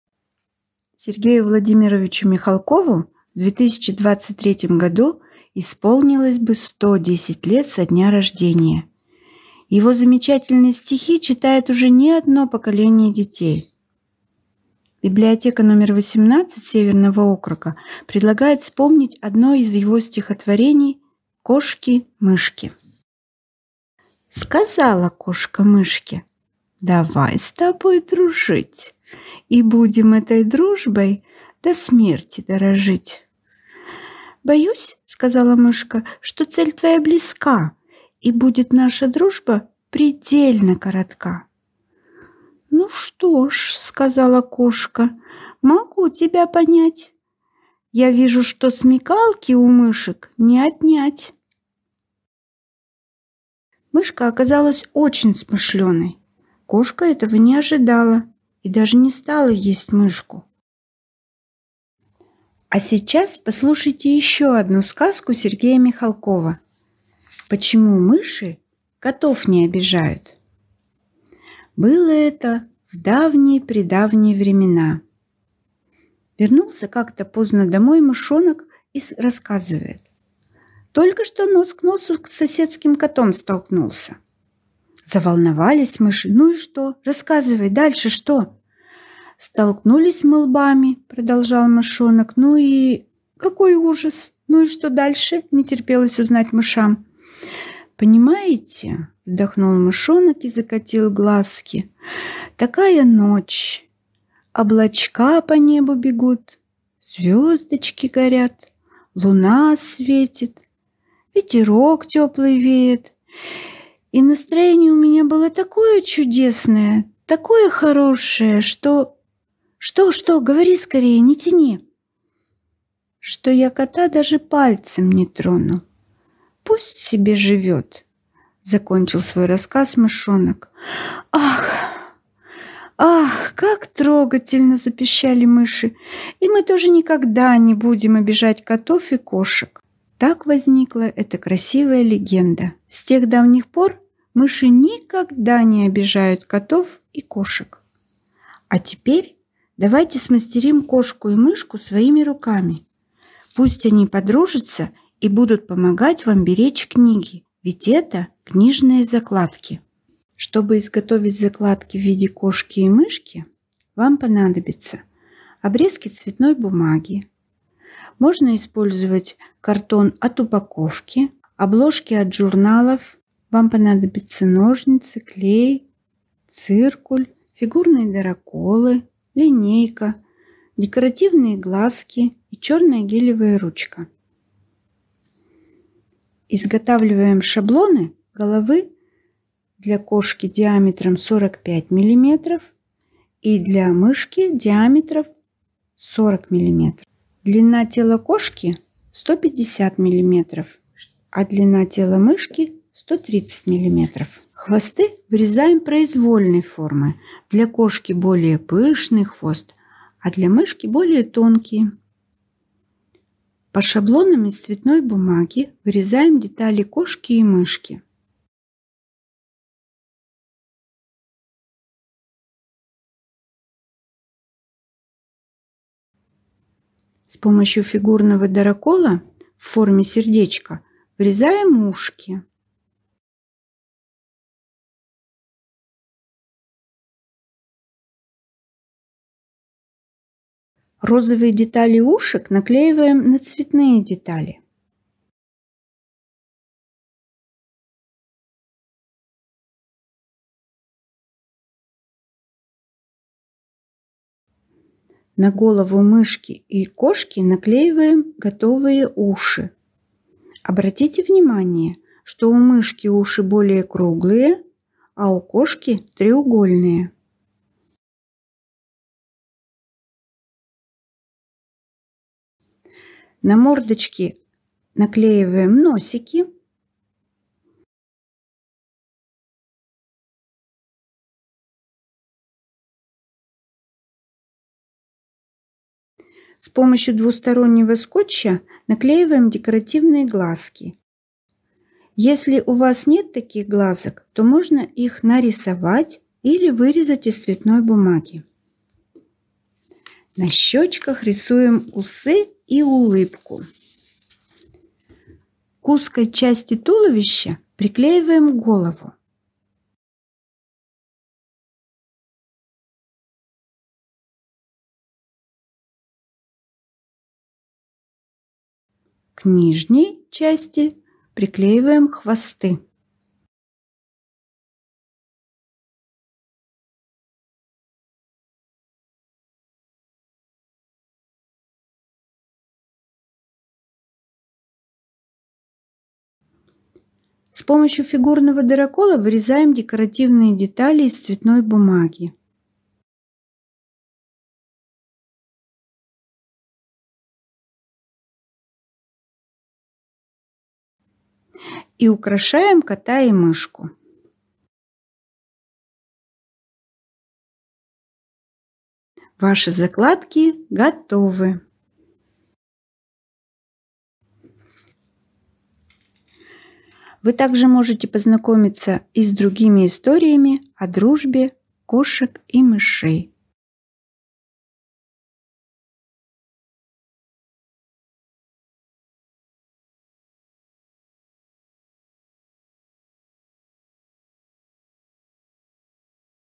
«Давай с тобой дружить», — сказала кошка мышке.
Библиотека № 18 Северного округа читает произведения Сергея Михалкова о дружбе кошек-мышек и показываете мастер-класс по изготовлению книжных закладок.